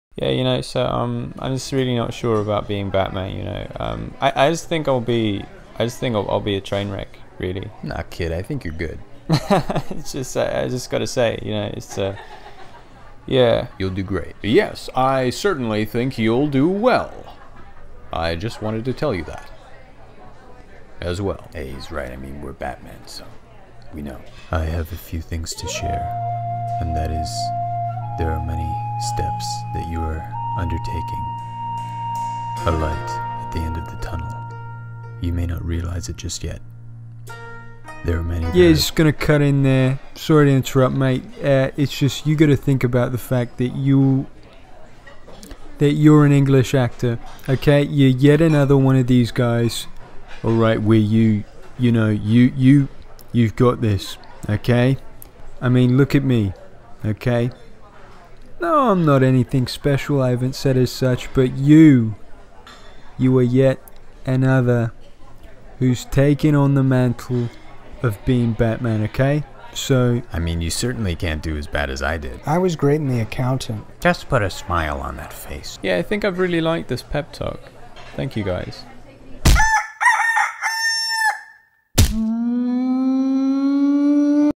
Robert Pattinson gets ADVICE from OTHER BATMAN
Robert gets a little pep talk from Christian Bale, Val Kilmer, George Clooney, Ben Affleck, Adam West, Michael Keaton, on how to play the role of batman

Robert doesn't have the confidence!

Voiced and written by @Nik_zutshi
Animated by @miggsytv